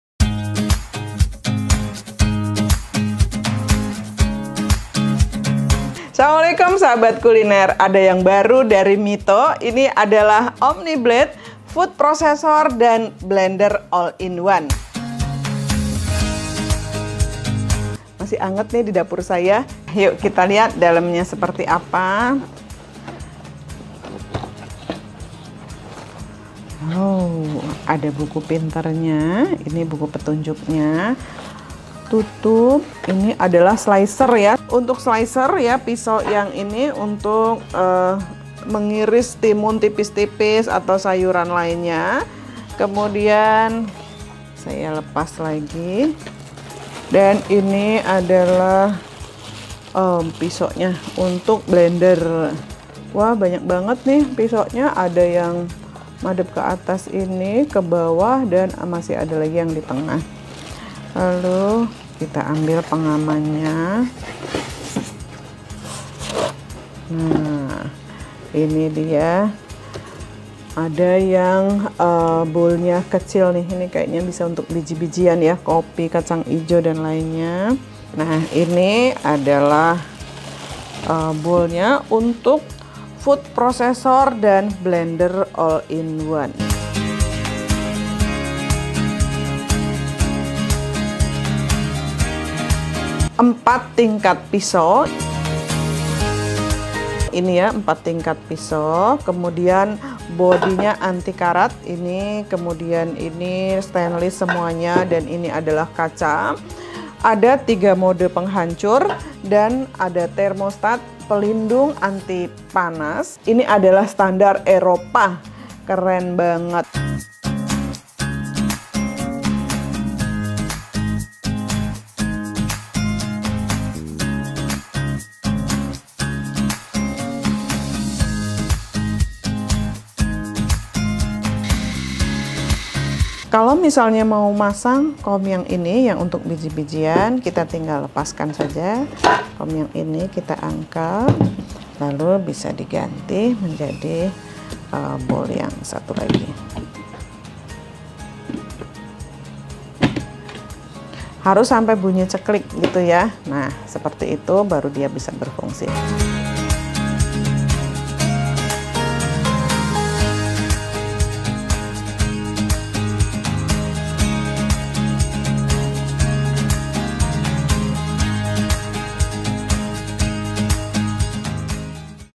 Unboxing Mito BL 100 II Food Processor dan Blender All in One
Blender dan Food processor BL100 MITO

Spesifikasi:

- Food processor dan Blender All in one (daging, buah, es, bumbu, dll)
- Kapasitas 1,5 liter gelas kaca
- Kapasitas 0,8 liter grinder stainless steel
- 8 mata pisau untuk hasil gilingan lebih halus
- Pisau pengiris
- BPA FREE
- Motor power full 20.000 rpm
- Daya 800watt
- Thermostat (pengaman motor tidak terbakar karena overheat)